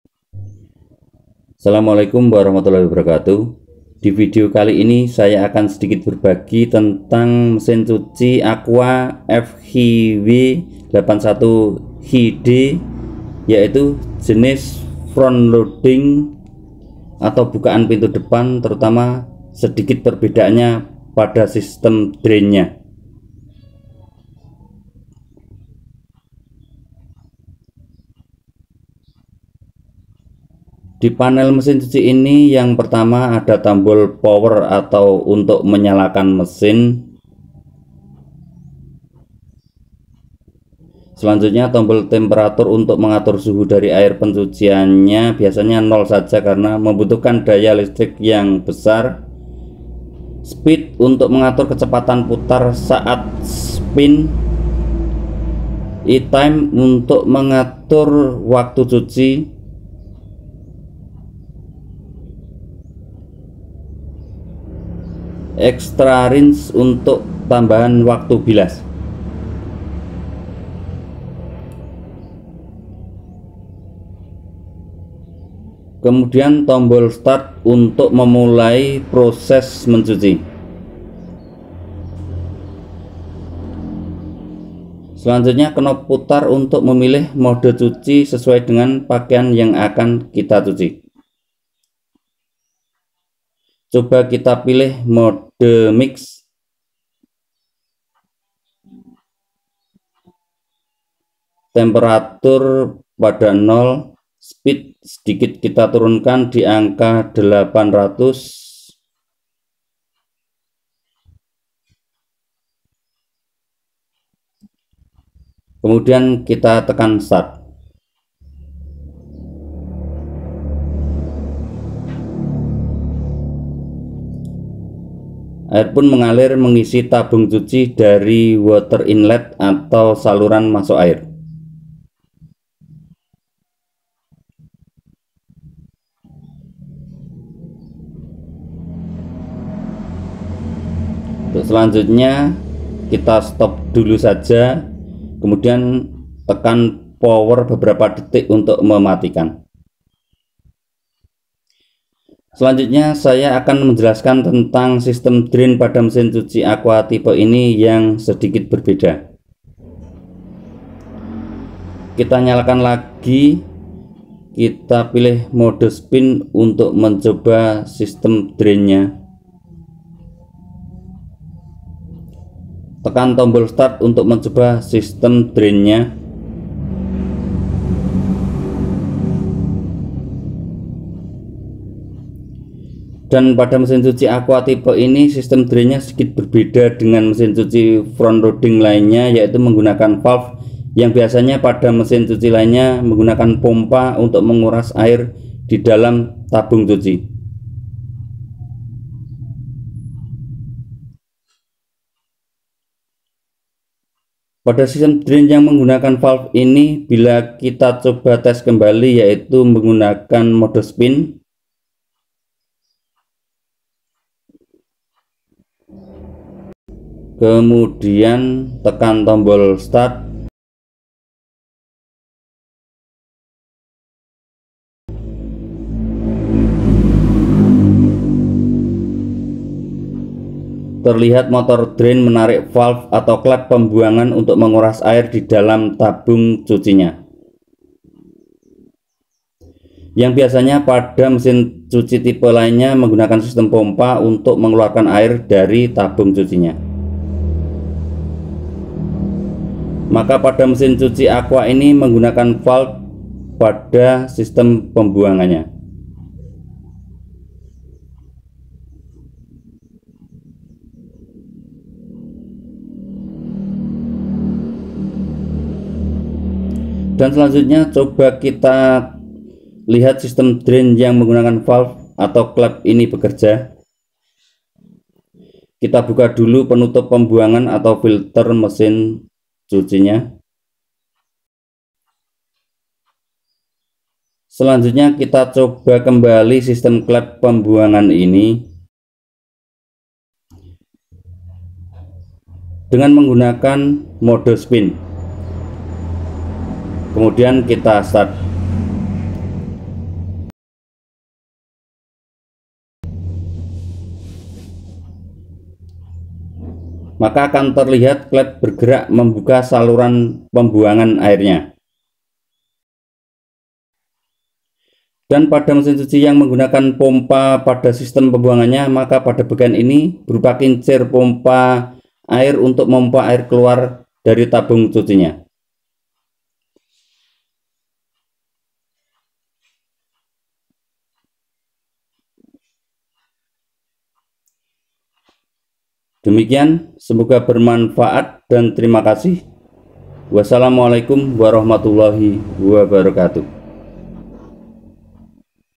MESIN CUCI AQUA FRONT LOADING FQW-81QD ,PERBEDAAN PADA BAGIAN DRAINNYA
MESIN CUCI AQUA FQW-81QD FRONT LOADING,PERBEDAAN PADA BAGIAN DRAINNYA YANG MENGGUNAKAN VALVE/KLEP.. DIBANDING MESIN CUCI FRONT LOADING PADA UMUMNYA YANG MENGGUNAKAN POMPA UNTUK MENGURAS AIR DARI TABUNG CUCI.